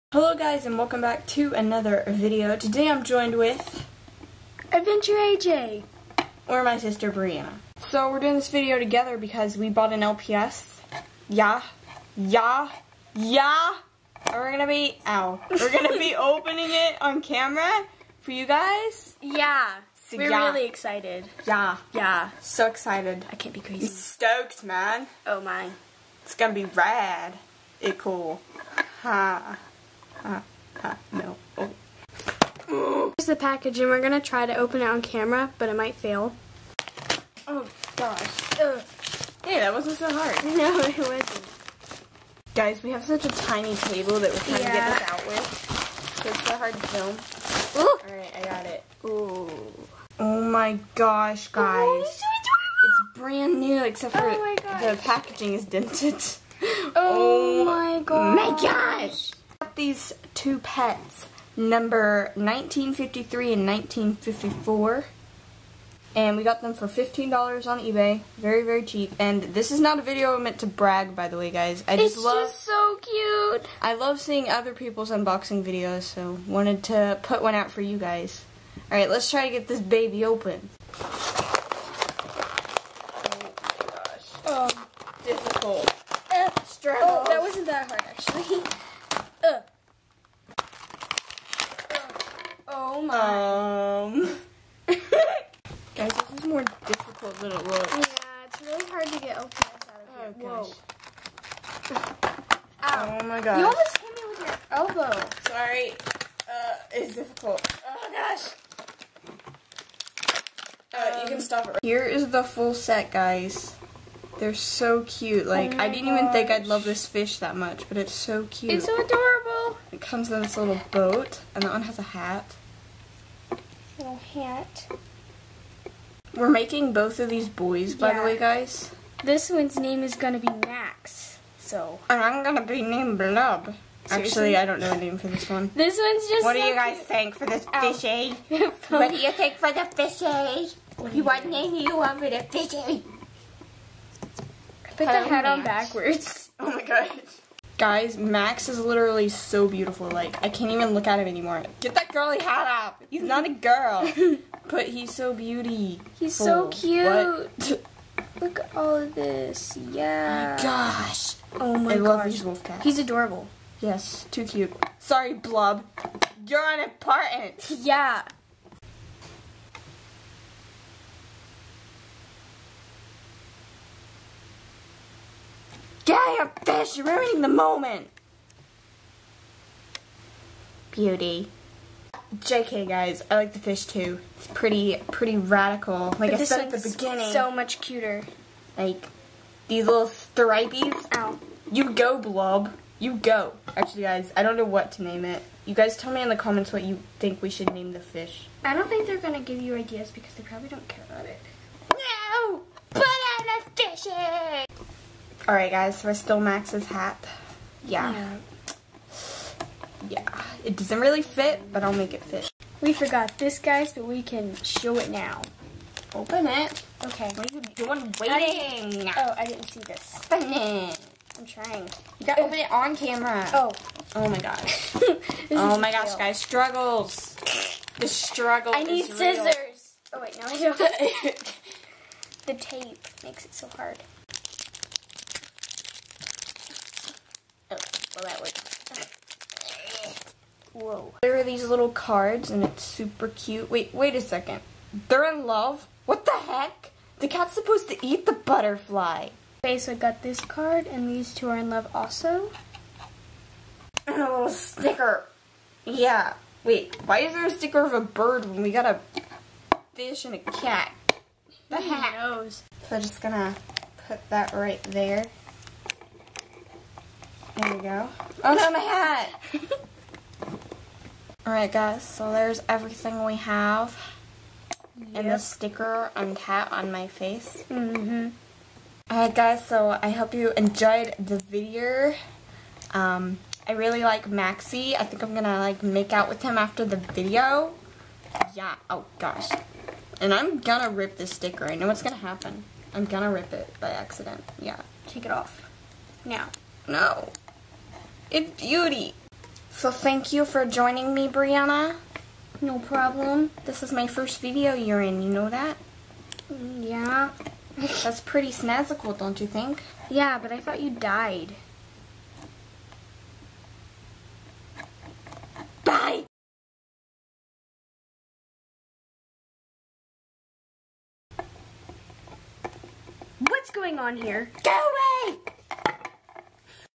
Unboxing My New Lps From Ebay! Ft. Adventure Aj
Hope you all enjoyed this little unboxing video! In this video I am joined with my sister, Adventure Aj! I am really happy with the lps that we got and am excited to make videos with them